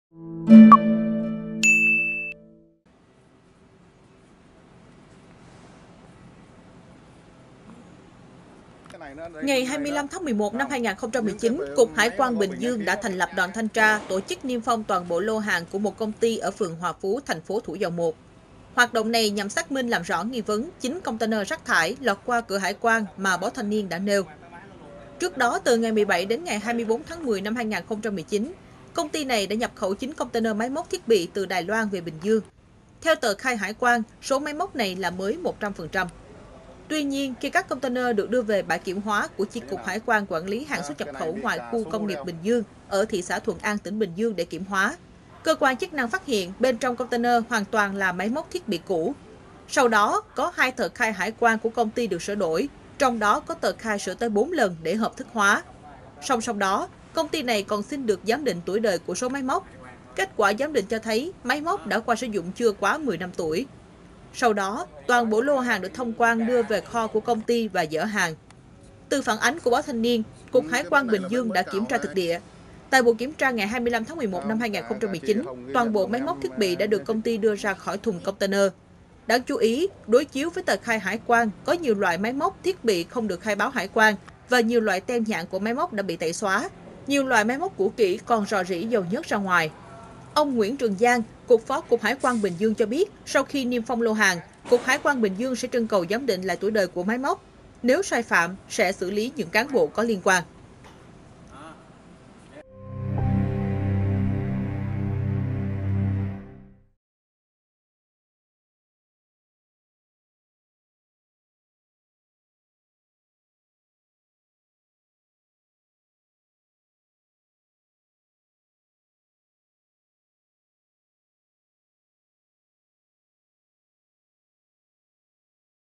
Kiểm tra thông tin 9 container “rác thải” lọt qua cửa hải quan
Ngày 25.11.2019, Cục Hải quan Bình Dương đã thành lập Đoàn thanh tra, tiến hành niêm phong toàn bộ lô hàng của một công ty ở phường Hoà Phú, thành phố Thủ Dầu Một. Hoạt động này nhằm để xác minh làm rõ nghi vấn 9 container “rác thải” lọt qua cửa hải quan mà Báo Thanh Niên đã nêu.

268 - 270 Nguyễn Đình Chiểu, Phường 6, Q.3, TP HCM